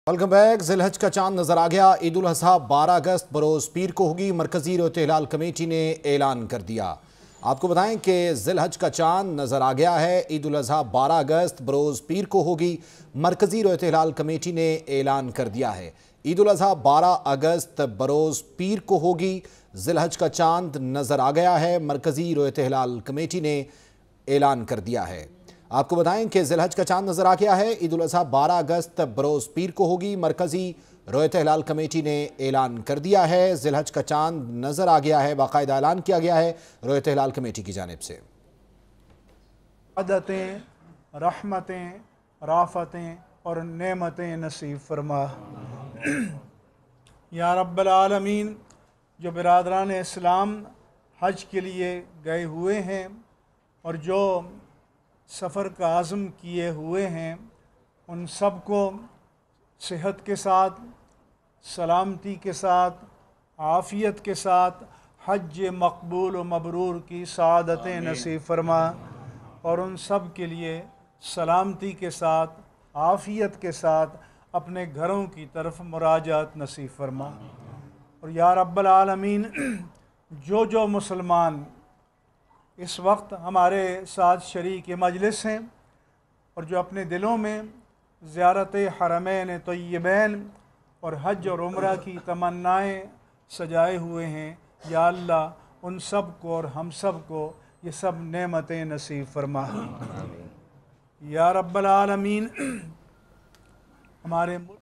Exclusive: Zil Hajj Moon Sighted, Eid-ul-Azha To Be Observed on 12th August | Lahore News HD
Exclusive: Zil Hajj Moon Sighted, Eid-ul-Azha To Be Observed on 12th August